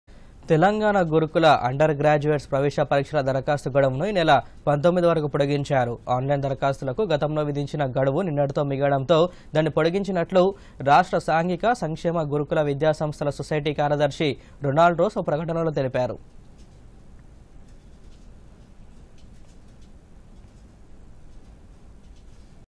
Telangana Gurukul undergraduate entrance exam extended to 19th of this month
తెలంగాణ గురుకుల అండర్‌ గ్రాడ్యుయేట్‌ ప్రవేశ పరీక్షల  దరఖాస్తు గడువును ఈ నెల 19 వరకు పొడిగించారు. ఆన్‌లైన్‌ దరఖాస్తులకు గతంలో విధించిన గడువు నిన్నటితో  ముగియడంతో దాన్ని పొడిగించినట్లు రాష్ట్ర సాంఘిక సంక్షేమ గురుకుల విద్యాసంస్థల సొసైటీ కార్యదర్శి రొనాల్డ్‌ రోస్ ఒక ప్రకటనలో  తెలిపారు.